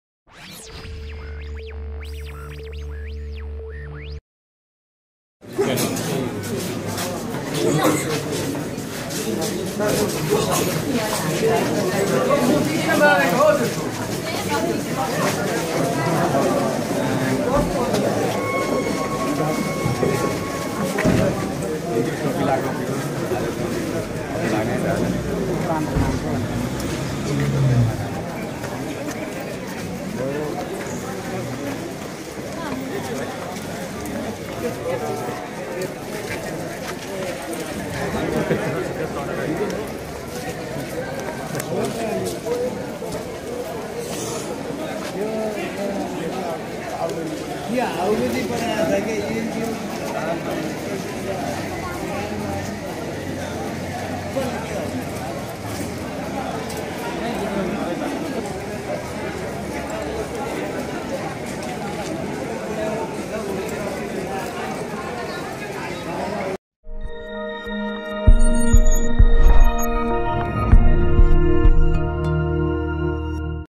मोरङ जिल्ला अदालतमा यसरी ल्याइयो गोरेलाई || Gore in Morang Court
३३ किलो सुन तस्करी प्रकरणका मुख्य नाइके मानिएका चूडामणि उप्रेती (गोरे) लाई बुधबार बयानका लागि मोरङ जिल्ला अदालत ल्याइएको थियो।